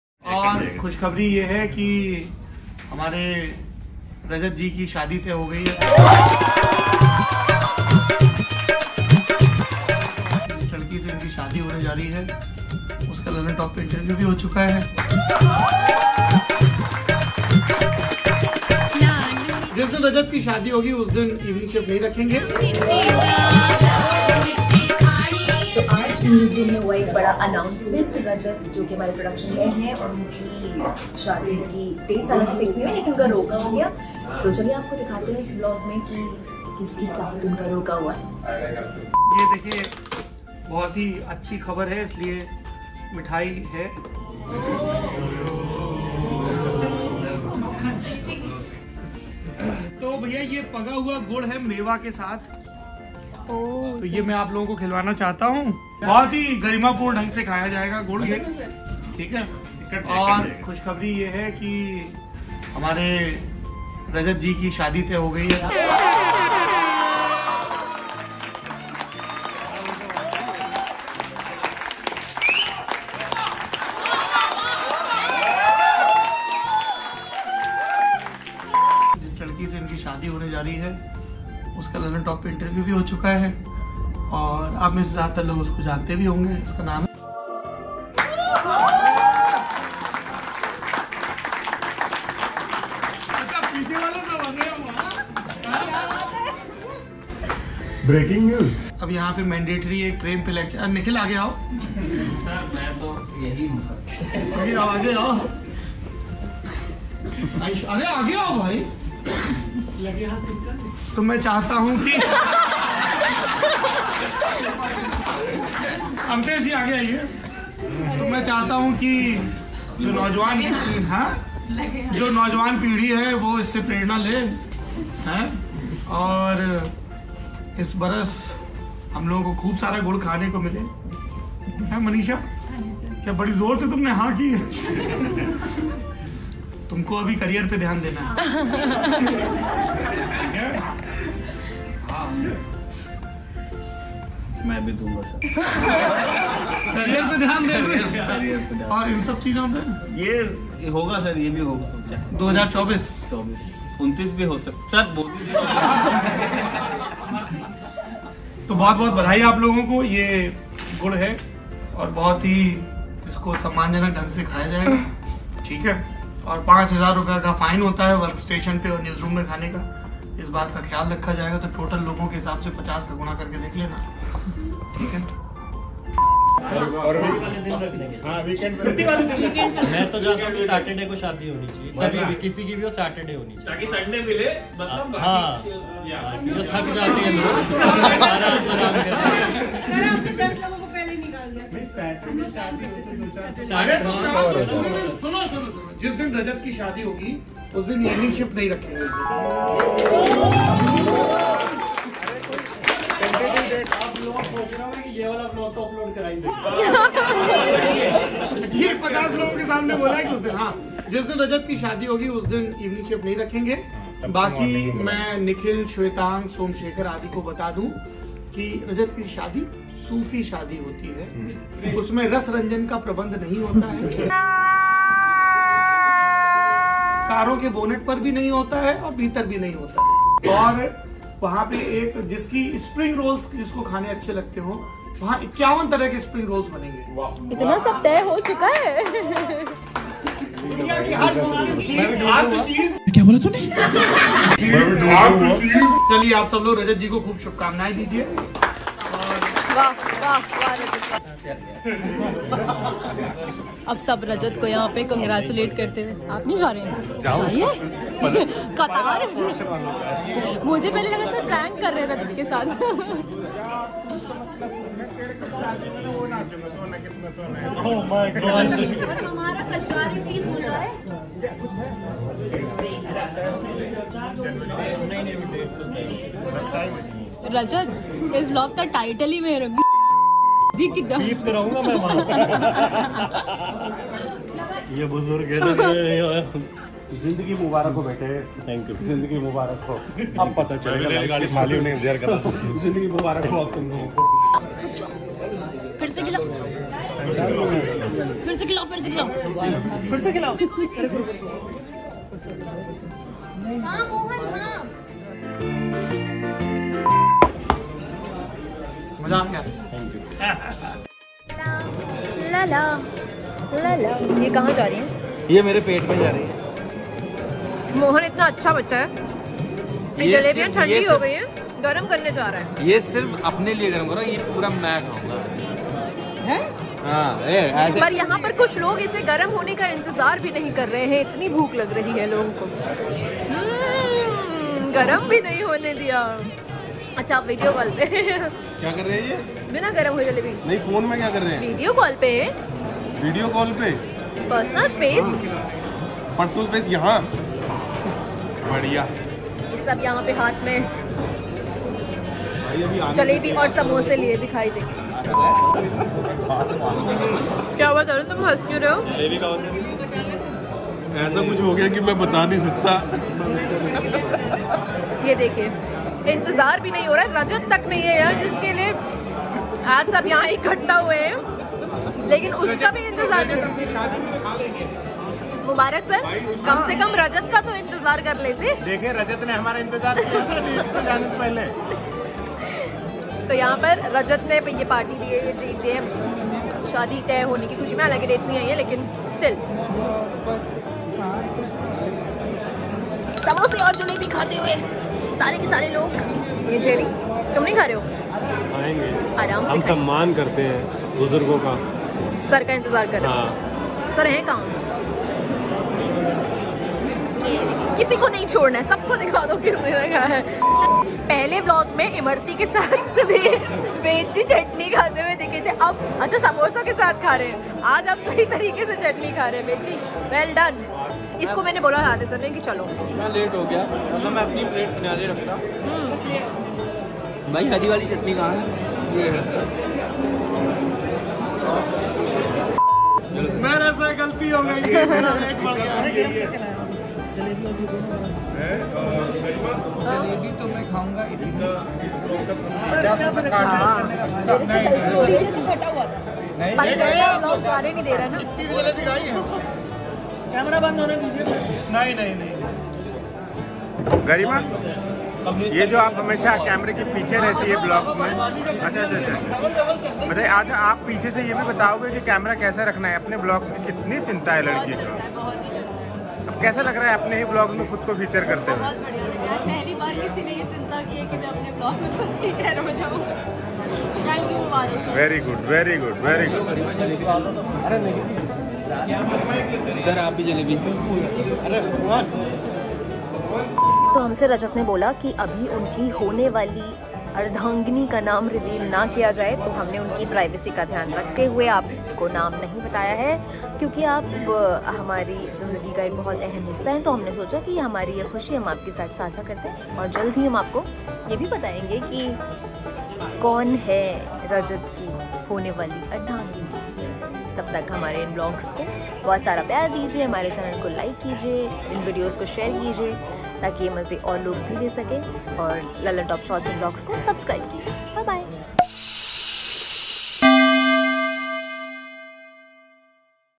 Saurabh Dwivedi ने Rajat Sain की शादी पर क्या बड़ा ऐलान कर दिया? | LT Insider | Vlog 17
Saurabh Dwivedi shared a happy news with the entire newsroom and the news is that Lallantop’s production head Rajat Sain is getting married. In our newsroom at Lallantop, we are all family and behave the same way. So we were all excited with this news. In this Vlog of LT Insider, know how we celebrated this special news at The Lallantop.

Edited By: Jeet